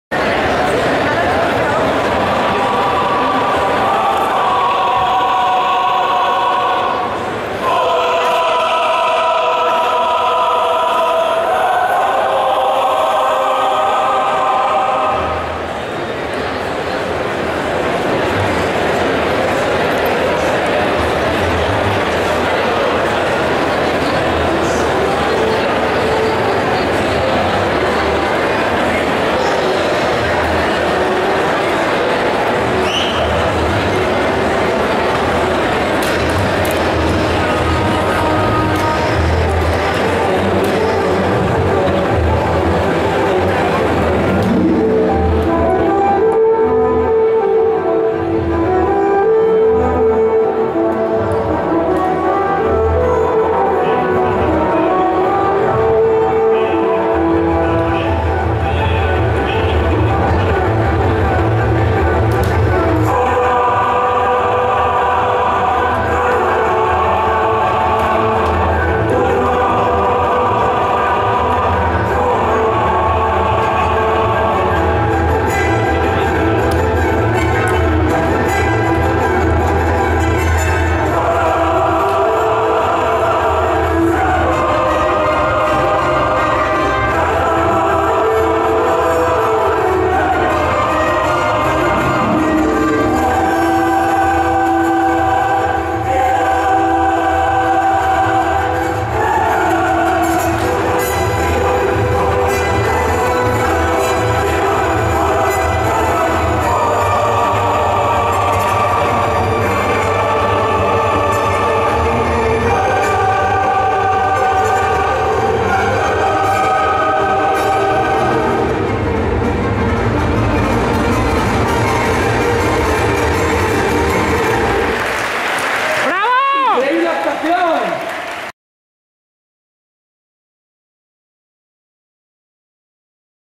Exhibición Cosplay - Akatsuki Córdoba - Weekend Play 2014
Exhibición de Cosplay en la 1ª Feria de Entretenimiento "Weekend Play" 2014 de Torremolinos, con parte del equipo de Akatsuki Córdoba en el escenario.